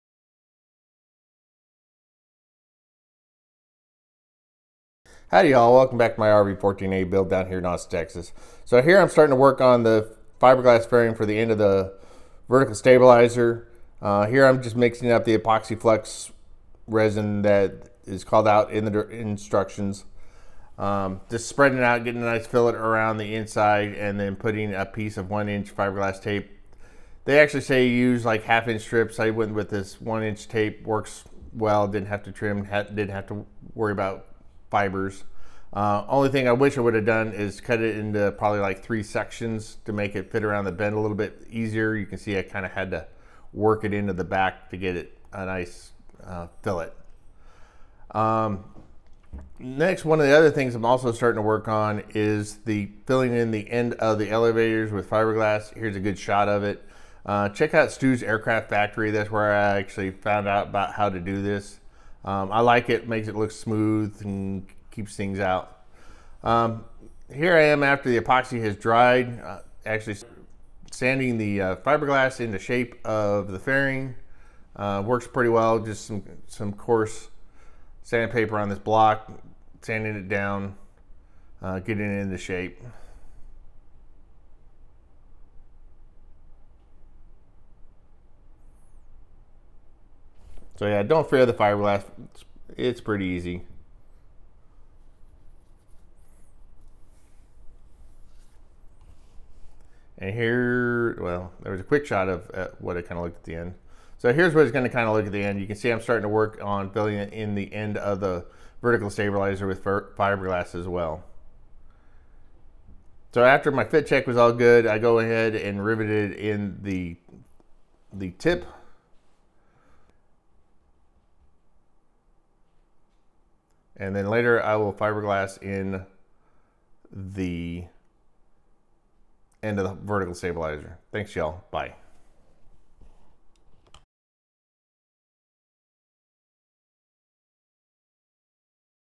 Van's RV-14A Vertical Stabilizer Tip
Installation of VS-109 Vertical stabilizer tip on Van's Aircraft RV-14A